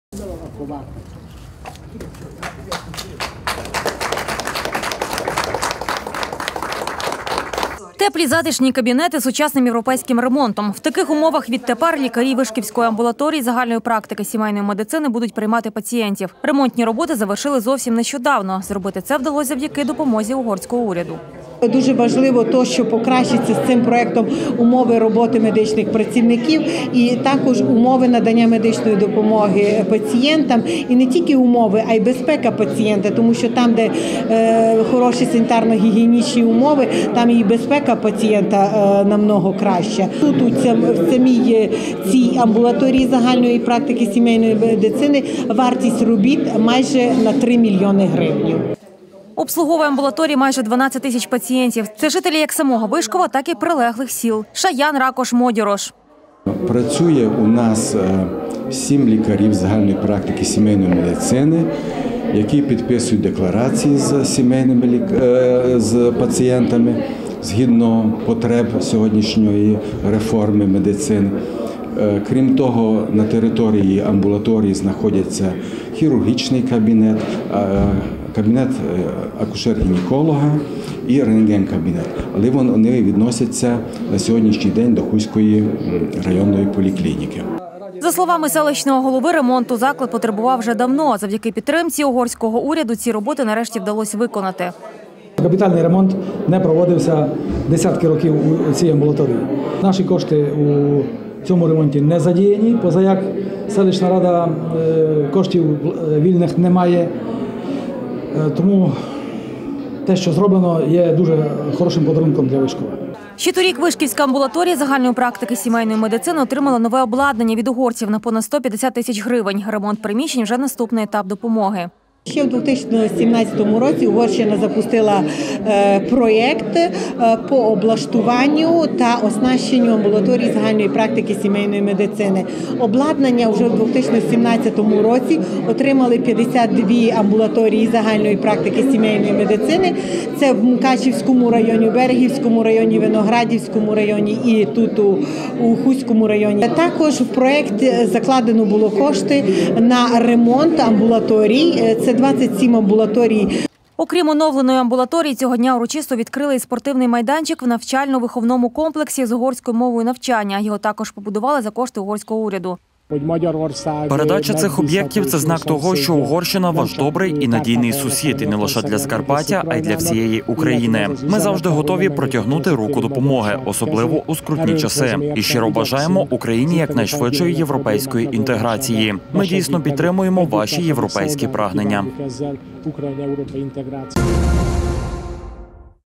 Оновлений об'єкт
Спортивний майданчик та оновлену амбулаторію відкрили у селищі Вишково на Хустщині. За кошти угорського уряду вдалося зробити ремонт в медзакладі, а в навчально-виховному комплексі побудувати сучасний майданчик для занять спортом.